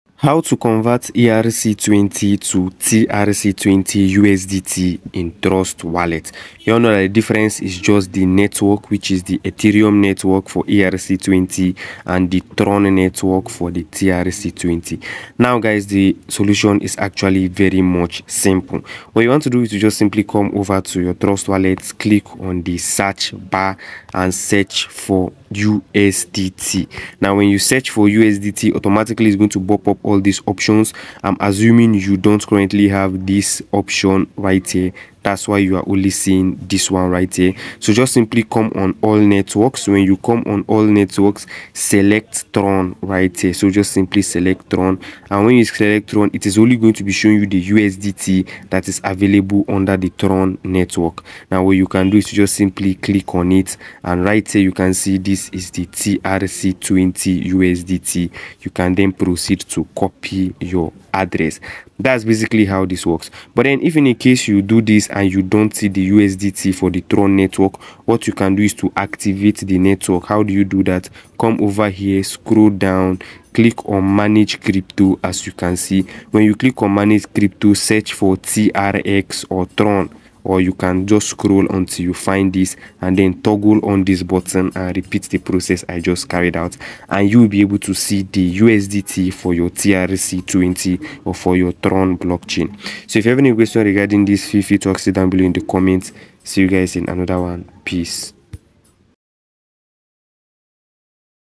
HOW TO CONVERT ERC20 TO TRC20 IN TRUST WALLET
Want to convert ERC20 tokens to TRC20 tokens in Trust Wallet? In this video, we’ll guide you through the process of converting your tokens across different blockchain networks using Trust Wallet. Learn how to use decentralized exchanges (DEXs) or bridges to swap your ERC20 tokens for TRC20 tokens securely and efficiently.

We’ll provide clear, step-by-step instructions to ensure a smooth and successful conversion, so you can manage your crypto assets across different networks with ease.